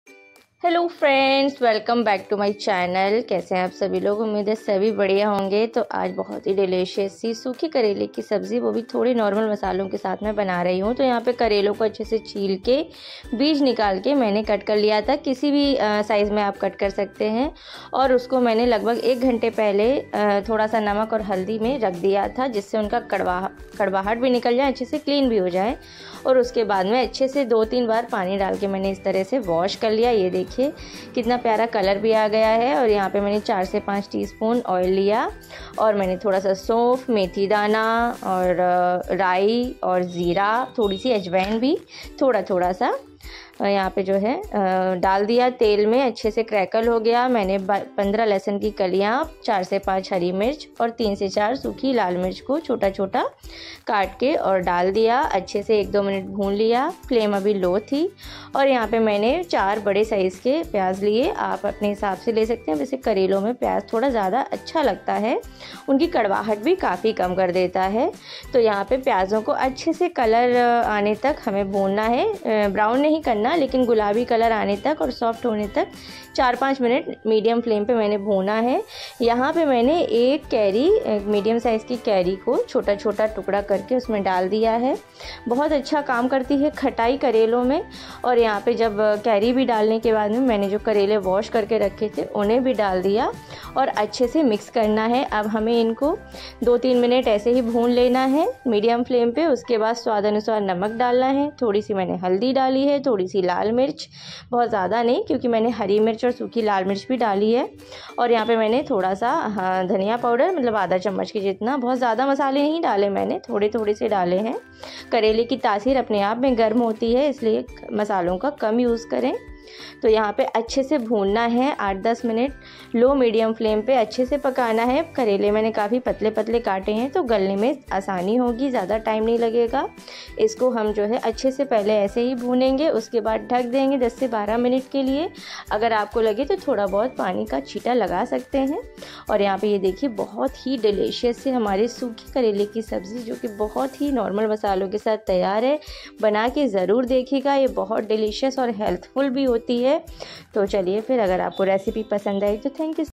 एक बार इस तरीके से करेले की सब्जी बनाइये, कहेँगे पहले क्यों नहीं बताया
करेले की सब्जी का ये तरीका देखके कहेँगे पहले क्यों नहीं बताया,